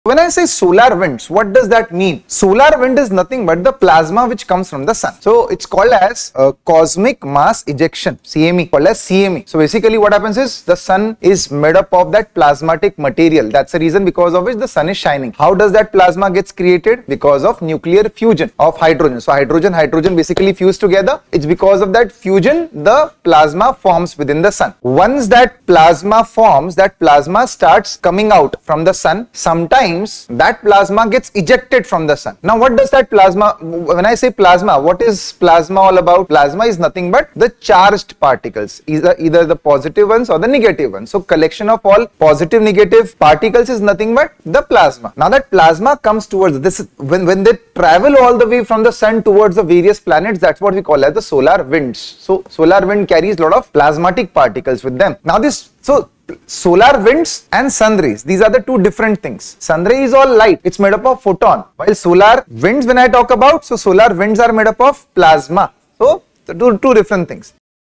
Are Solar Winds & Solar Rays Same? By Rohit Lodha Sir
Are Solar Winds & Solar Rays Same?

Are solar winds and solar rays the same? Many confuse these two phenomena, but they’re fundamentally different. Solar winds are streams of charged particles (protons, electrons) ejected from the Sun’s corona, while solar rays refer to photons emitted via nuclear fusion in the Sun’s core.

Solar winds, traveling at 250–750 km/s, shape Earth’s magnetosphere and trigger auroras. Solar rays (sunlight) are electromagnetic radiation, providing energy for life and solar panels. Coronal Mass Ejections (CMEs)—massive plasma bursts—are distinct from the steady solar wind.

Understanding these differences clarifies their impacts: solar winds disrupt satellites, while solar rays drive climate and renewable energy.